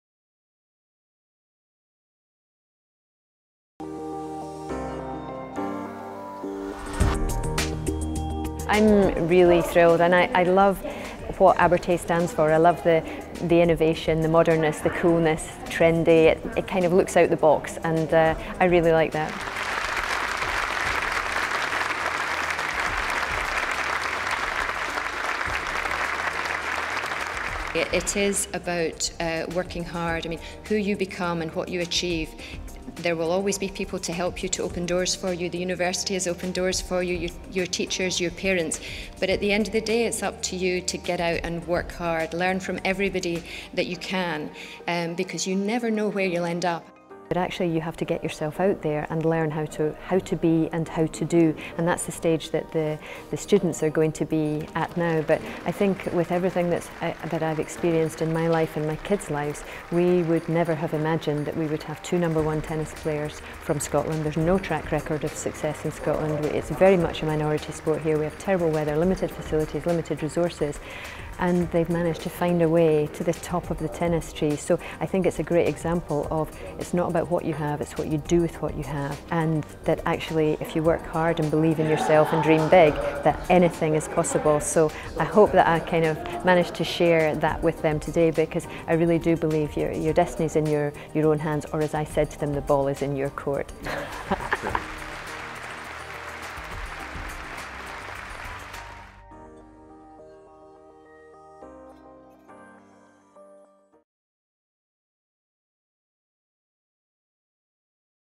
Interview with Judy Murray, Abertay Honorary Graduate 2016
Wise words from Abertay Honorary Graduate Judy Murray, Tennis Coach and Doctor of Education